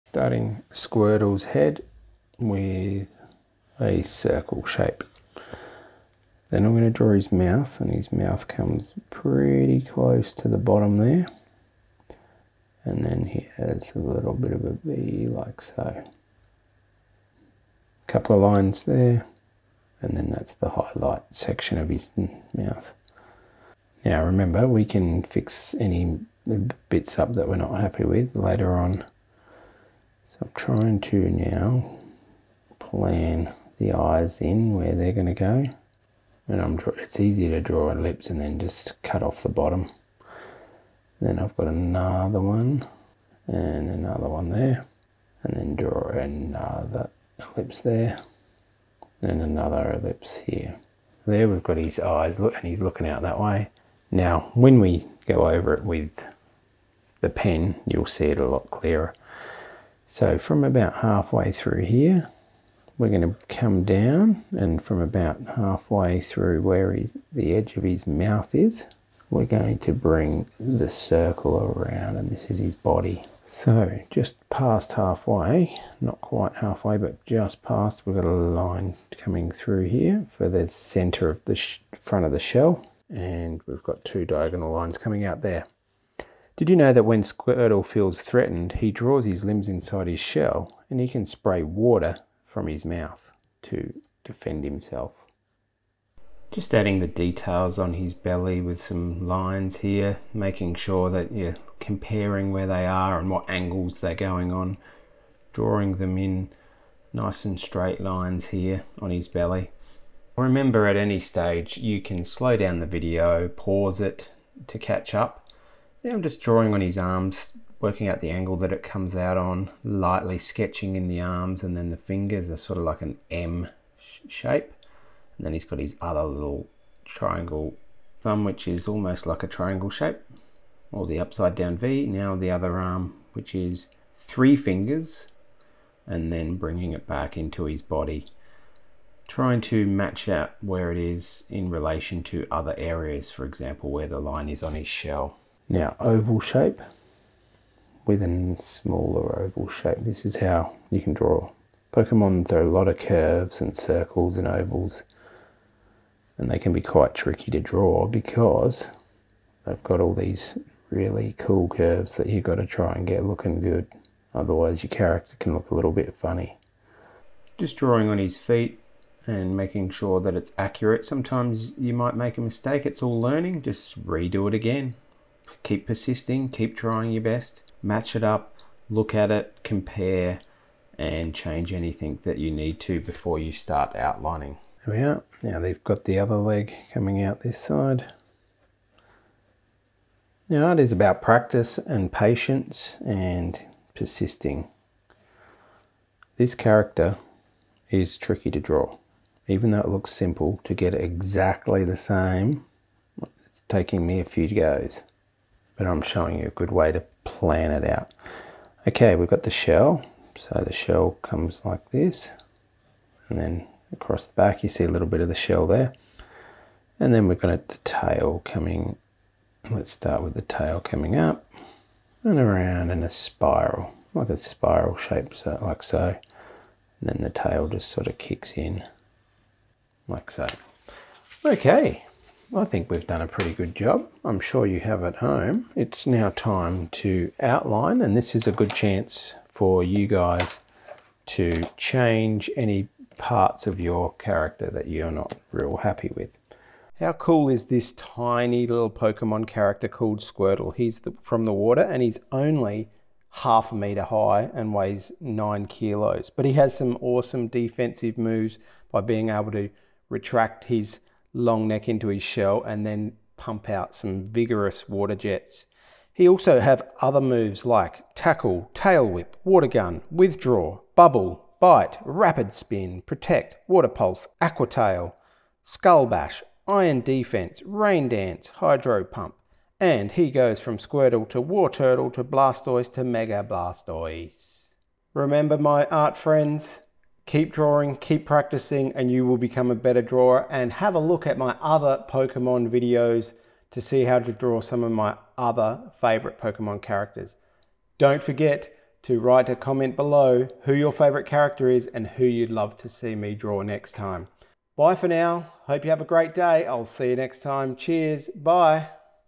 How to Draw Squirtle - Tiny Turtle Pokemon Video
Learn how to draw the very cute tiny turtle Pokemon character called Squirtle in easy to follow step by step instructions. Let me know what other Pokemon characters you would like to see me draw.

Shoutout to Hudson for your suggestion.

COPIC MARKERS USED:
E33, E99, RV14, R85, YR31, B97, B93, N5